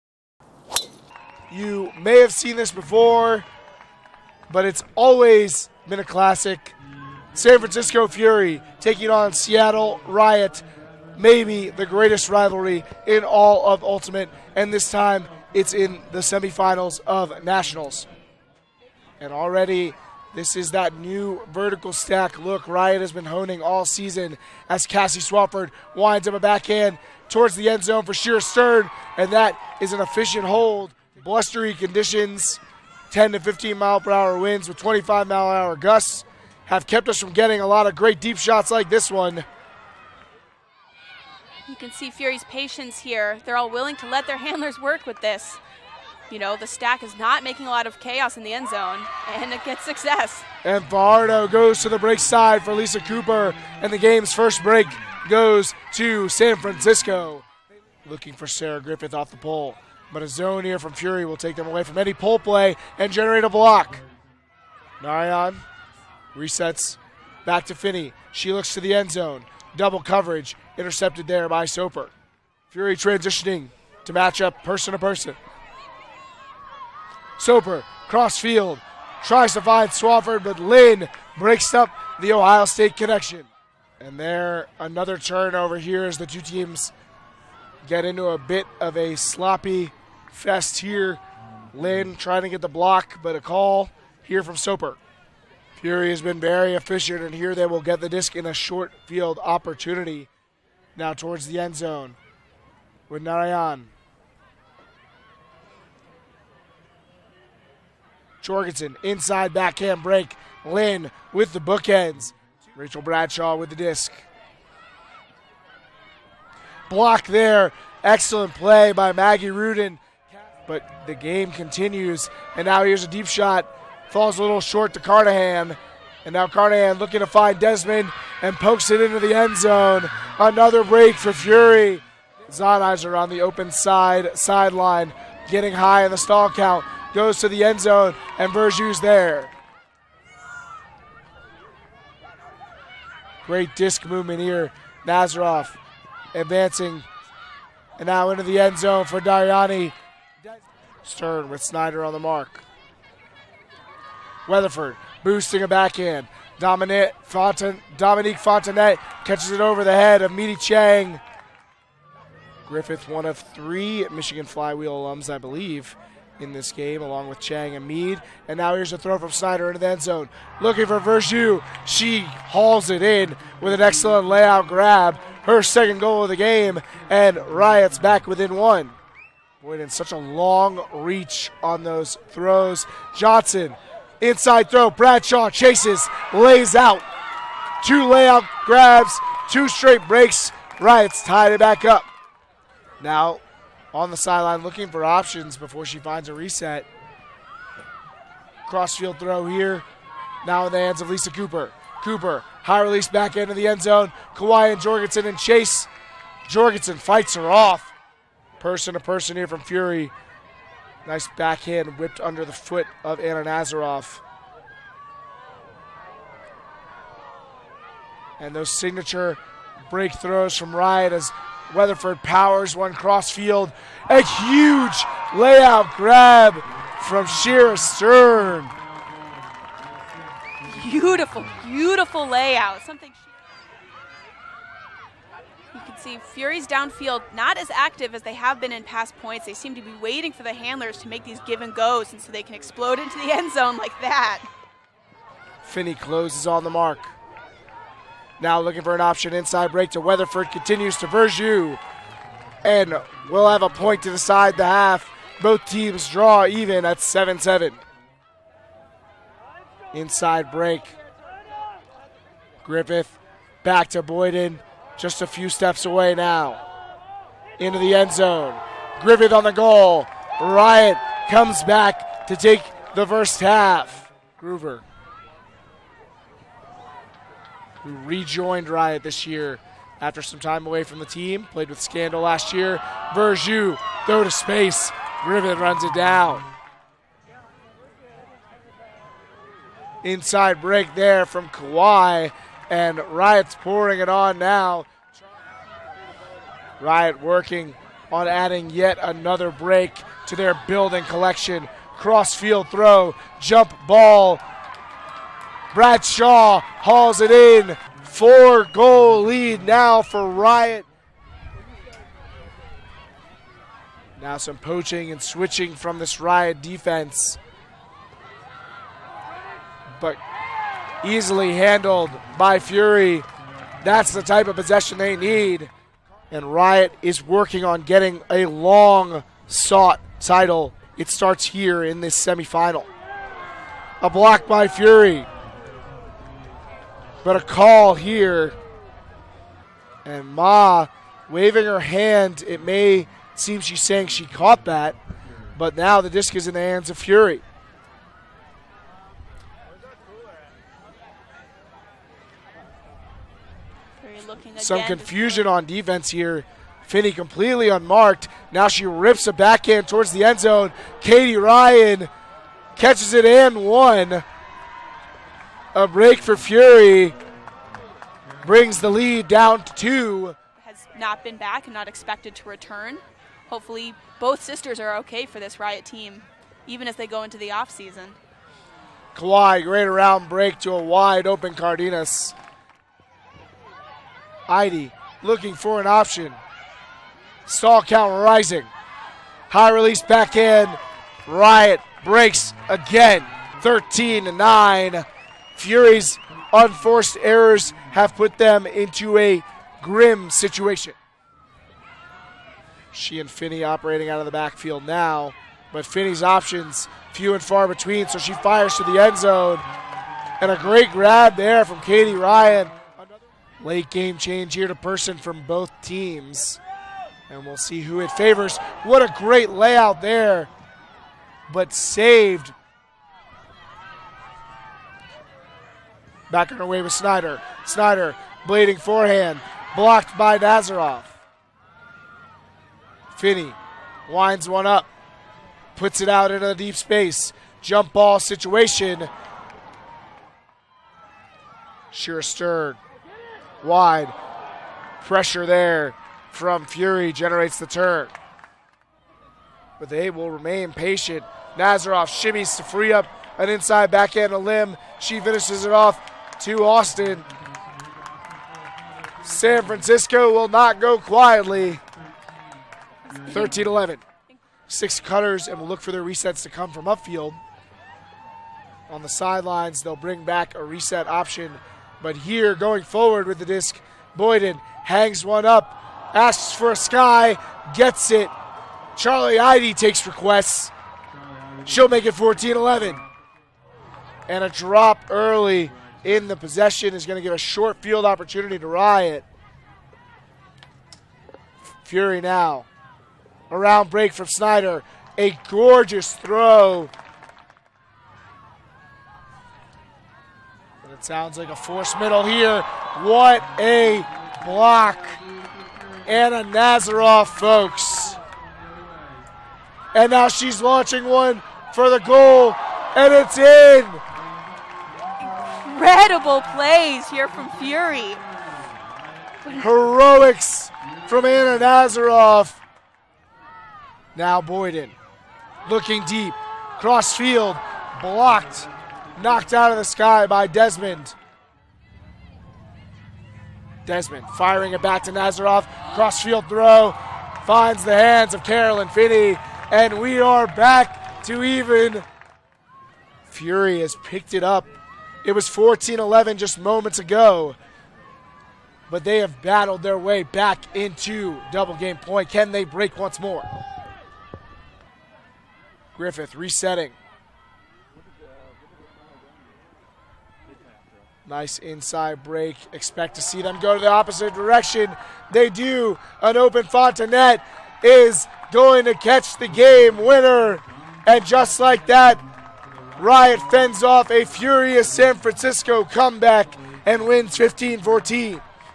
FURY vs RIOT | 2016 Club National Championship Semifinal | FULL GAME HIGHLIGHTS
Full Game Highlights from the 2016 Club Nationals Semifinal between San Francisco Fury and Seattle Riot.